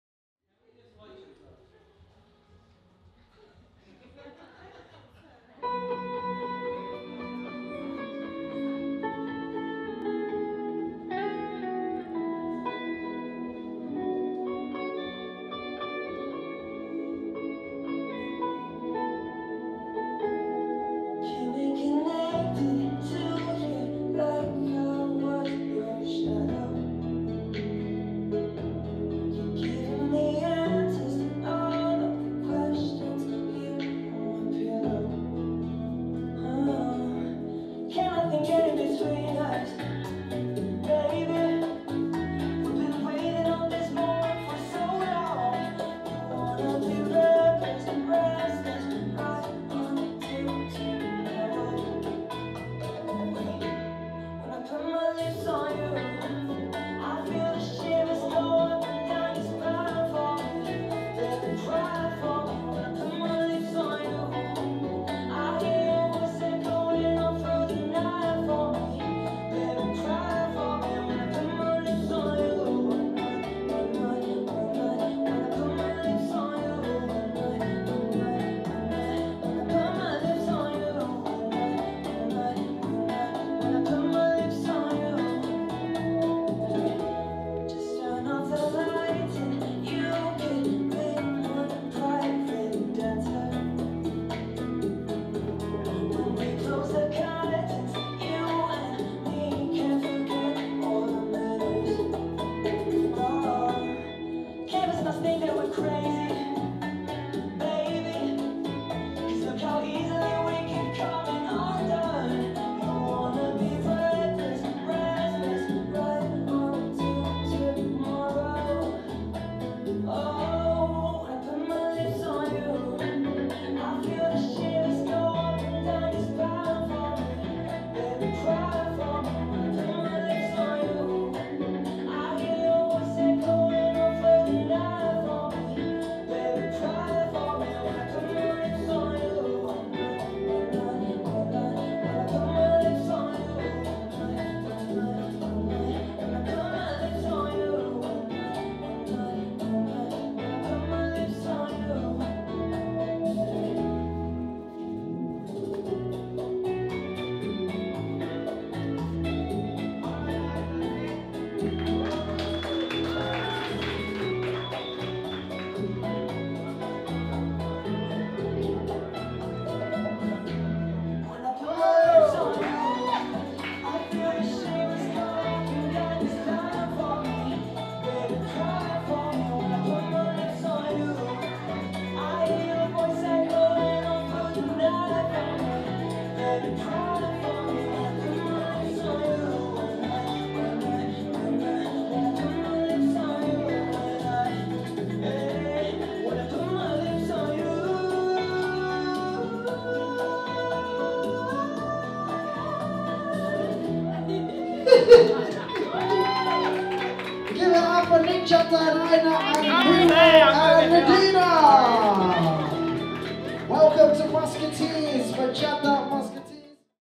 Nicholas Theodorou & Aina Akmalieva Bachata Demo 2022.02.27. by Bachatamusketeers
Bachata Advanced demo performed by Nicholas Theodorou & Aina Akmalieva at Be At One Regent Street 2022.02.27. by Bachatamusketeers

Be At One Regent Street

Address: 10 Beak St, London W1F 9RD
5 mins from Piccadilly Circus!